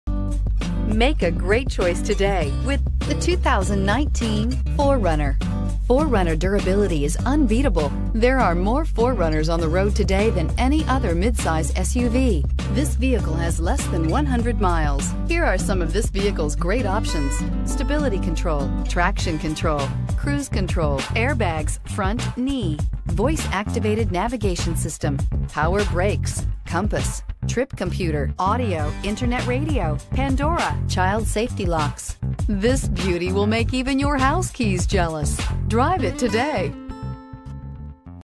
2019 Toyota 4Runner Torrance CA TT19S385
2019 Toyota 4Runner SR5 Premium

Dch Toyota of Torrance
2909 Pacific Coast Highway

For a smoother ride, opt for this 2019 Toyota 4Runner SR5 Premium with a backup camera, Bluetooth, a navigation system, braking assist, a power outlet, hill start assist, stability control, traction control, premium sound system, and a MP3 player.  It has a 6 Cylinder engine.  This one's on the market for $40,143.  With a crash test rating of 4 out of 5 stars, everyone can feel secure.  For a good-looking vehicle from the inside out, this car features a suave midnight black met exterior along with a sand beige interior.  Interested? Call today and take it for a spin!